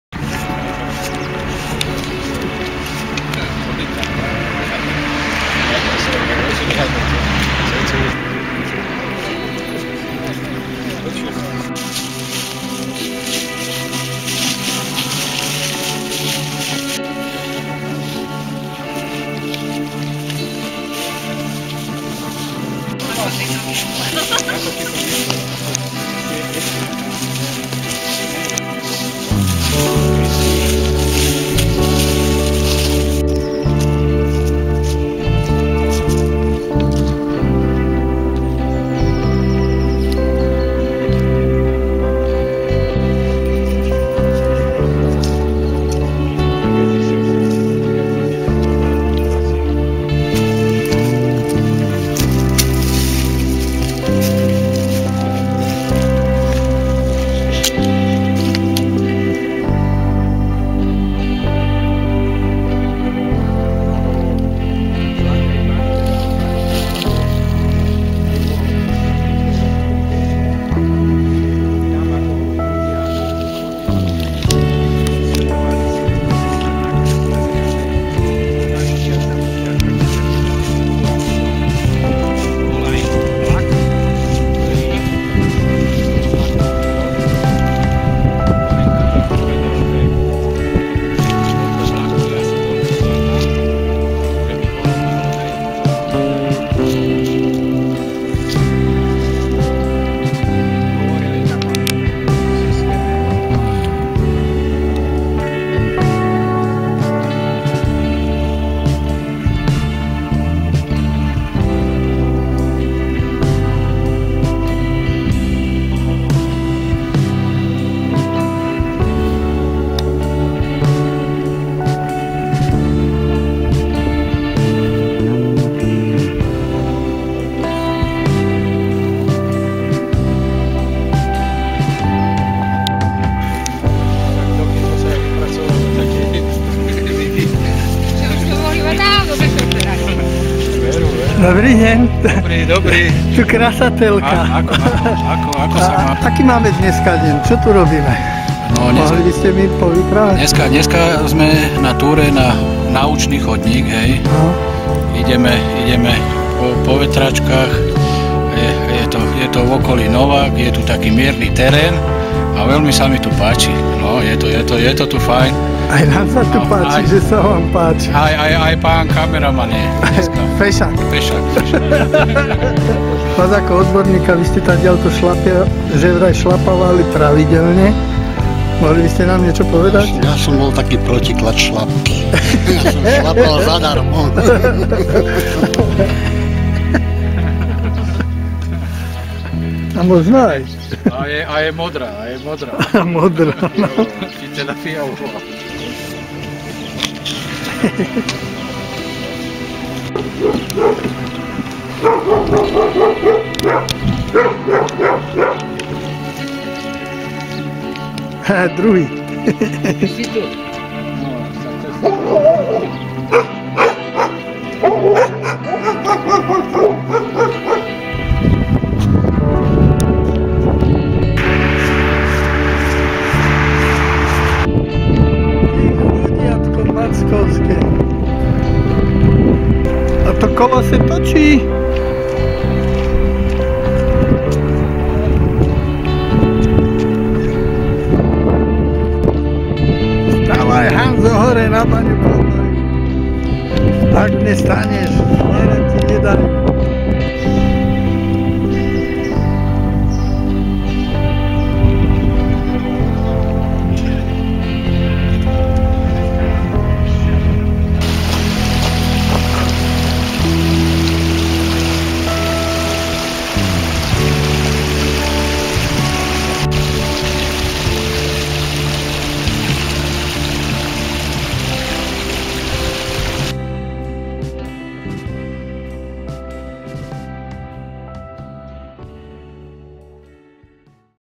Bansky naúčný chodník Nováky 12. 3. 2017
Prechádzka lesom nelesom začínajúcou jarou...